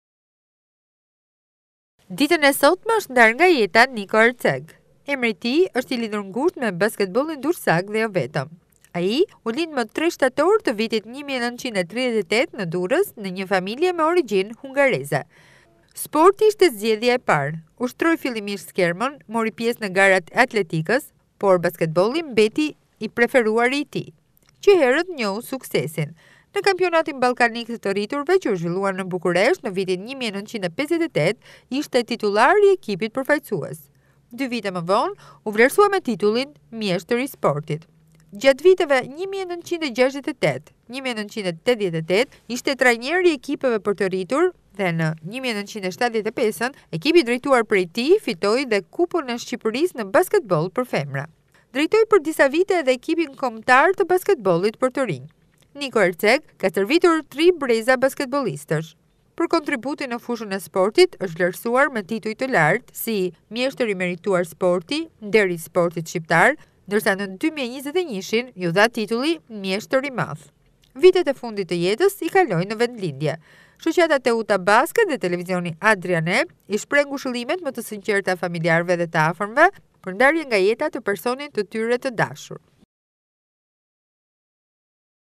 Ndahet nga jeta Niko Herczegh, figura e nderuar e basketbollit durrsak
Ndahet nga jeta Niko Herczegh, figura e nderuar e basketbollit durrsak, i vlerësuar me titullin "Mjeshtër i Madh"